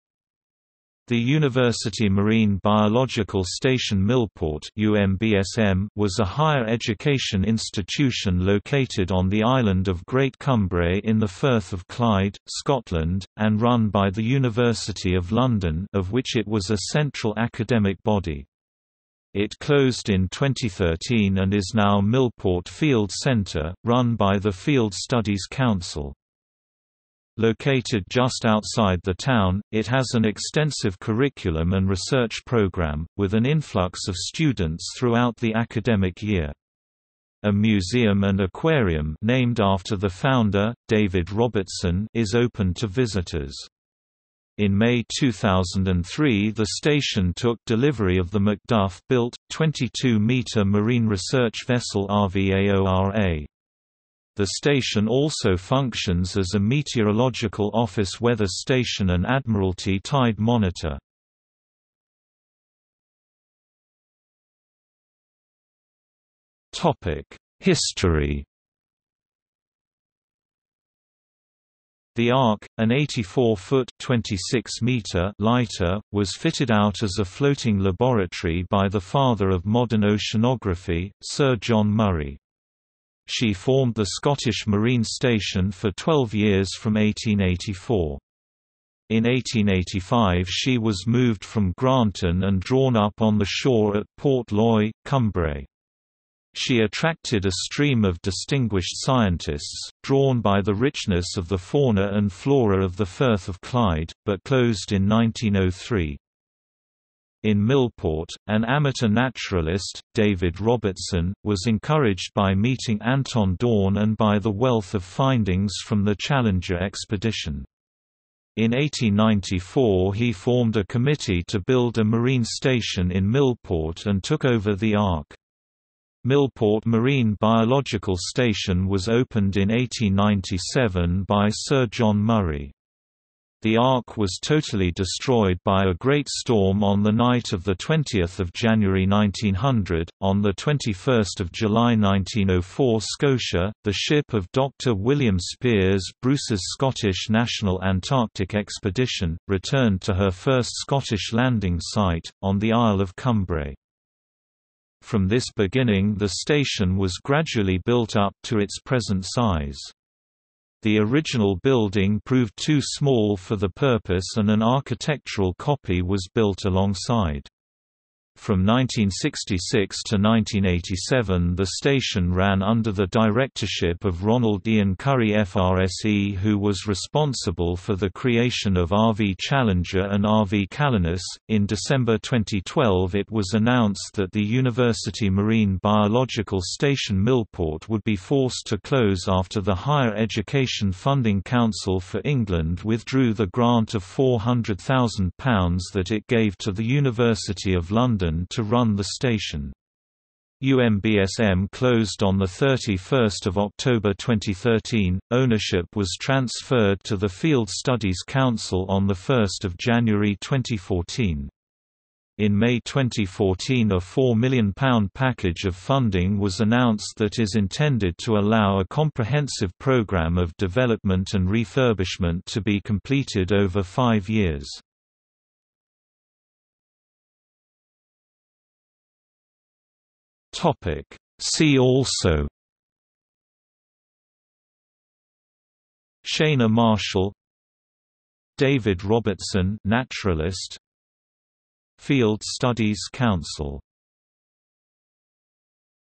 University Marine Biological Station Millport | Wikipedia audio article
00:01:03 1 History
00:03:46 2 See also

- Socrates

SUMMARY
The University Marine Biological Station Millport (UMBSM) was a higher education institution located on the island of Great Cumbrae in the Firth of Clyde, Scotland, and run by the University of London (of which it was a central academic body). It closed in 2013 and is now Millport Field Centre, run by the Field Studies Council.
Located just outside the town, it has an extensive curriculum and research programme, with an influx of students throughout the academic year. A Museum and Aquarium (named after the founder, David Robertson) is open to visitors. In May 2003 the station took delivery of the Macduff-built, 22-metre marine research vessel RV Aora. The station also functions as a Meteorological Office Weather Station and Admiralty Tide Monitor.